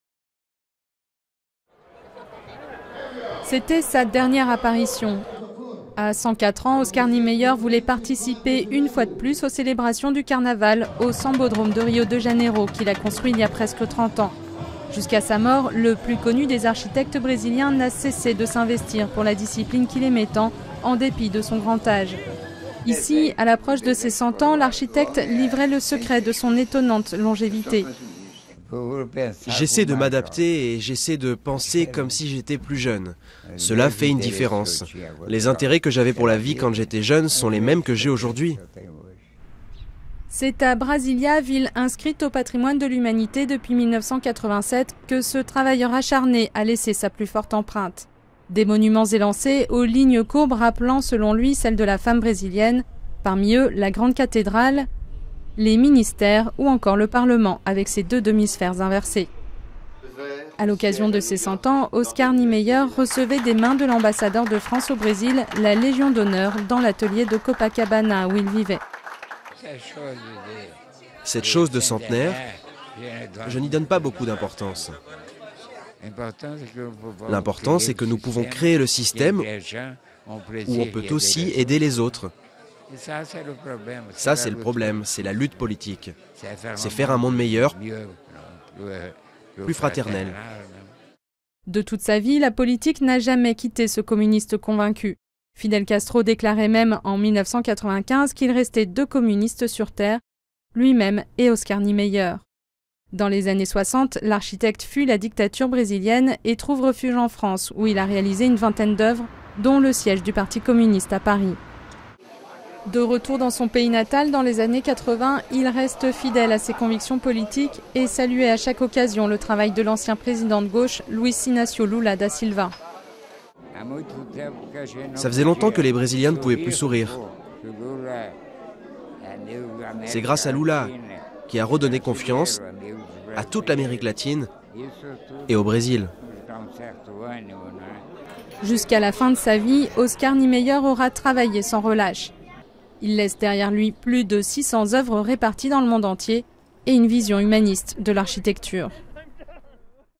Décès d'Oscar Niemeyer, le père de l'archictecture brésilienne
Le Brésilien Oscar Niemeyer est décédé à l'âge de 104 ans. Le père de l'architecture brésilienne aura révolutionné l'architecture moderne. Il laisse derrière lui plus de 600 œuvres dans le monde et encore une vingtaine de projets en cours au Brésil et à l'étranger. PORTRAIT. Durée: 02:20